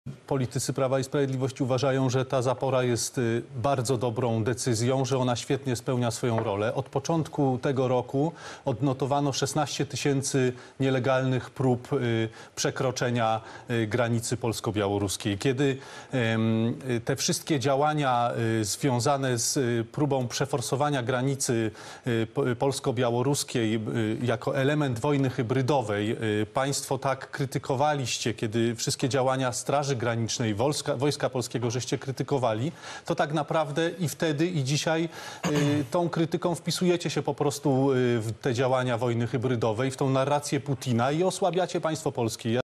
PO osłabia Polskę
Platforma Obywatelska krytykując działania rządu PiS na granicy polsko-białoruskiej, wpisuje się w narracje Putina i osłabia tym samym państwo polskie - Wicemarszałek senator  Marek Pęk.